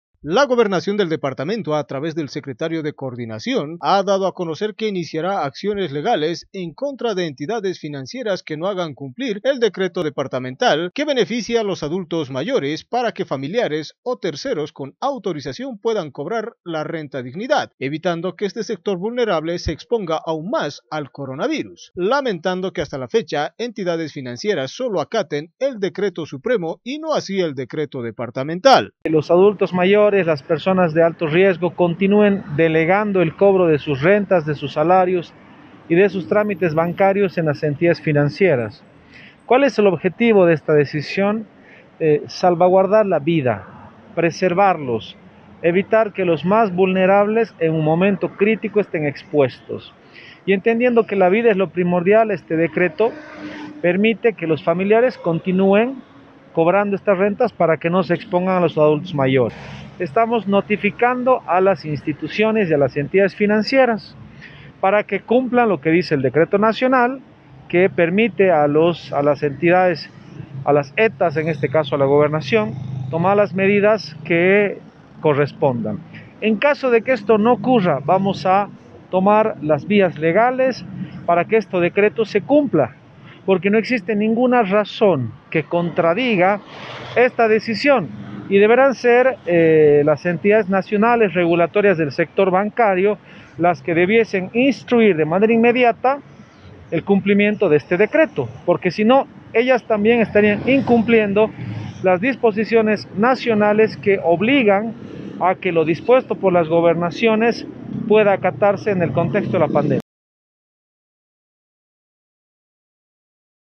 GOBERNACIÓN ANUNCIA PROCESOS PARA HACER RESPETAR EL DECRETO
WALDEMAR PERALTA – SECRETARIO DEPARTAMENTAL DE COORDINACIÓN
El secretario de coordinación de la Gobernación lamento que entidades financieras no acaten el decreto departamental para que ancianos deleguen a sus familiares el cobro de la renta dignidad. Por lo que anuncian procesos legales.